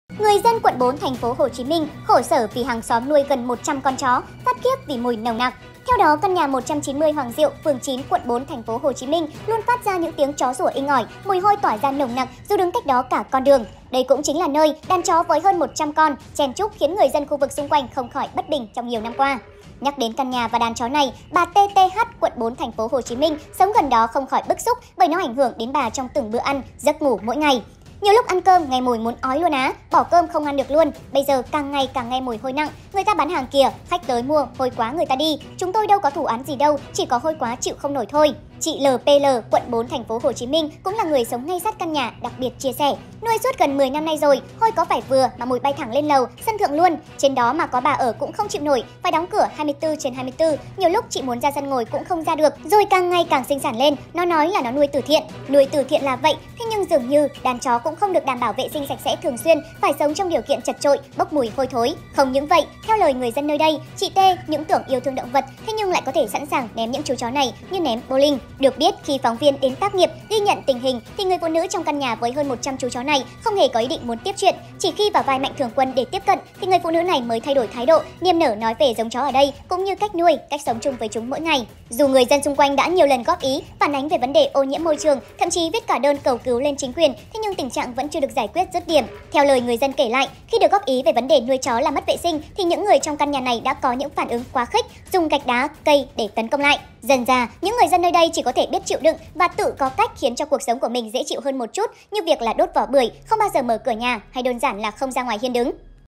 Nuôi gần 100 con chó khiến hàng xóm Phát khiếp vì mùi hôi nồng nặc
Kênh BÁO&THỦ liên tục cập nhật các tin tức mới nhất trong ngày !